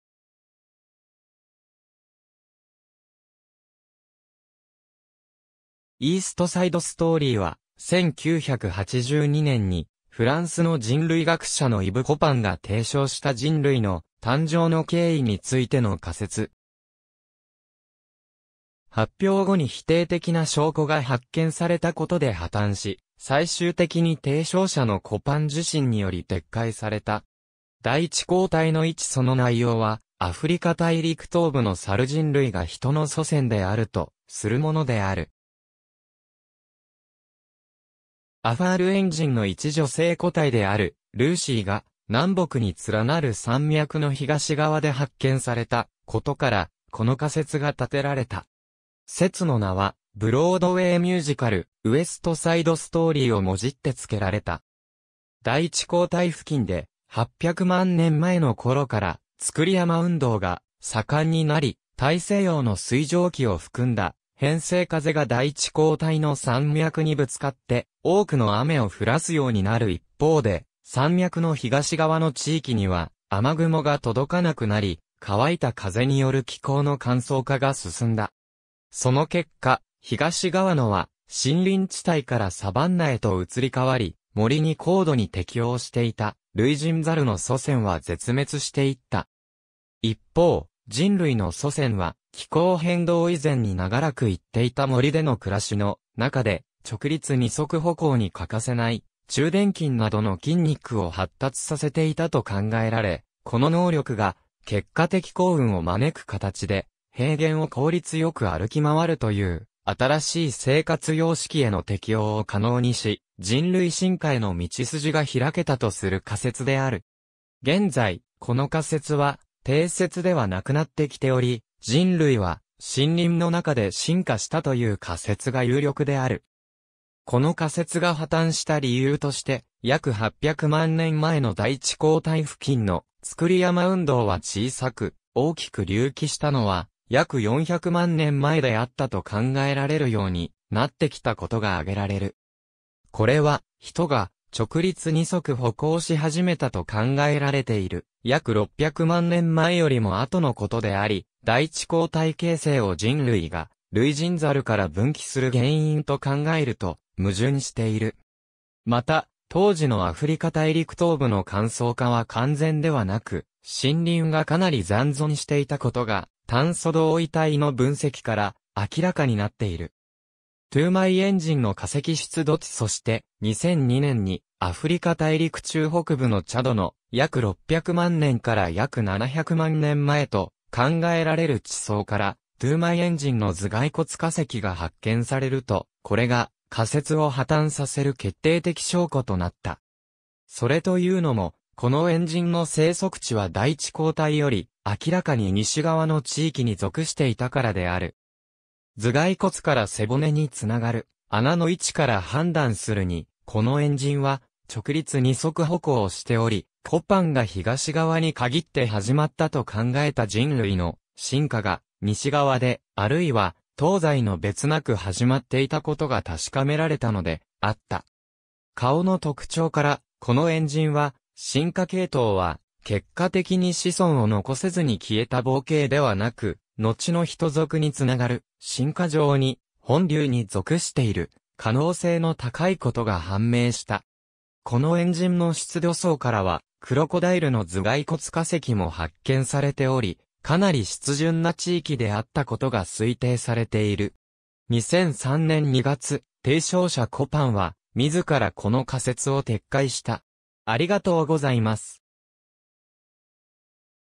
イーストサイドストーリー
イーストサイドストーリー（英: East Side Story）は、1982年にフランスの人類学者のイブ・コパンが提唱した人類の誕生の経緯についての仮説。
発表後に否定的な証拠が発見されたことで破綻し、最終的に提唱者のコパン自身により撤回された。
大地溝帯の位置 その内容は、アフリカ大陸東部の猿人類がヒトの祖先であるとするものである。
アファール猿人（アウストラロピテクス・アファレンシス）の一女性個体である“ルーシー”が、南北につらなる山脈の東側で発見されたことから、この仮説が立てられた。
説の名はブロードウェイ・ミュージカル『ウエストサイドストーリー』をもじって付けられた。
大地溝帯付近で、800万年前の頃から造山運動が盛んになり、大西洋の水蒸気を含んだ偏西風が大地溝帯の山脈にぶつかって多くの雨を降らすようになる一方で、山脈の東側の地域には雨雲が届かなくなり、乾いた風による気候の乾燥化が進んだ。
その結果、東側のは森林地帯からサバンナへと移り変わり、森に高度に適応していた類人猿の祖先は絶滅していった。
一方、人類の祖先は、気候変動以前に永らく行っていた森での暮らしのなかで、直立二足歩行に欠かせない中臀筋などの筋肉を発達させていたと考えられ（※類人猿の骨盤の化石から推定）、この能力が結果的幸運を招く形で、平原を効率よく歩き回るという新しい生活様式への適応を可能にし、人類進化への道筋が拓けたとする仮説である。
現在、この仮説は定説ではなくなってきており、人類は森林の中で進化したという仮説が有力である。
この仮説が破綻した理由として、約800万年前の大地溝帯付近の造山運動は小さく、大きく隆起したのは約400万年前であったと考えられるようになってきたことが挙げられる。
これはヒトが直立二足歩行し始めたと考えられている約600万年前よりも後のことであり、大地溝帯形成を人類が類人猿から分岐する原因と考えると矛盾している。
また、当時のアフリカ大陸東部の乾燥化は完全ではなく、森林がかなり残存していたことが炭素同位体の分析から明らかになっている。
トゥーマイ猿人の化石出土地 そして、2002年にアフリカ大陸中北部のチャドの約600万年から約700万年前と考えられる地層からトゥーマイ猿人（サヘラントロプス・チャデンシス）の頭蓋骨化石が発見されると、これが、仮説を破綻させる決定的証拠となった。
それと言うのも、この猿人の生息地は大地溝帯より明らかに西側の地域に属していたからである。
頭蓋骨から背骨につながる孔の位置から判断するに、この猿人は直立二足歩行をしており、コパンが東側に限って始まったと考えた人類の進化が、西側で、あるいは東西の別なく始まっていたことが確かめられたのであった。
顔の特徴から、この猿人は進化系統は「結果的に子孫を残せずに消えた傍系」ではなく、のちのヒト属につながる進化上に“本流”に属している可能性の高いことが判明した。
この猿人の出土層からはクロコダイルの頭蓋骨化石も発見されており、かなり湿潤な地域であったことが推定されている。
2003年2月、提唱者コパンは、自らこの仮説を撤回した。